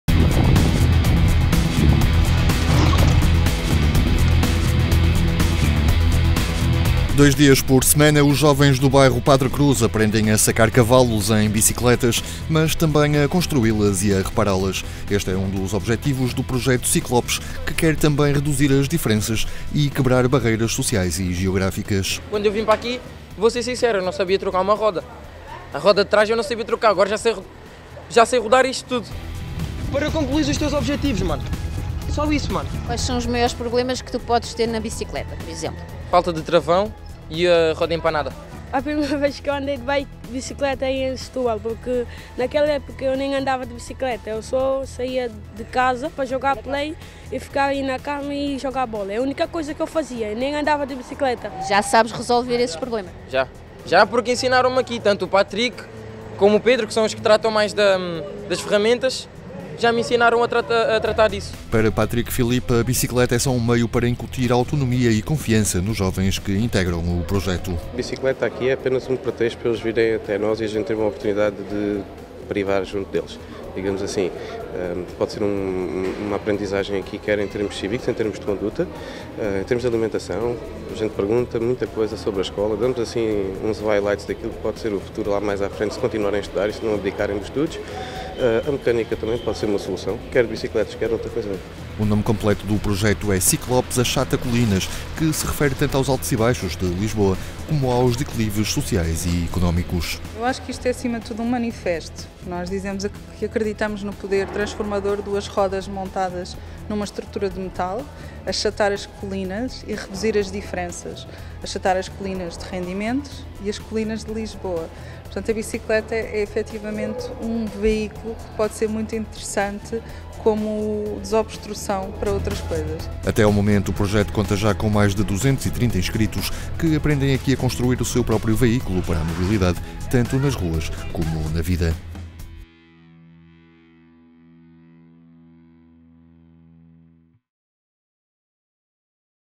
Miúdos do Bairro Padre Cruz sacam cavalinhos nas 'bikes'
Achatar as colinas de Lisboa e reduzir as diferenças, quebrando barreiras sociais e geográficas, é o objetivo do projeto Ciclopes que leva miúdos do Bairro Padre Cruz, dois dias por semana, a pegar na bicicleta e a ‘sacar’ cavalinhos.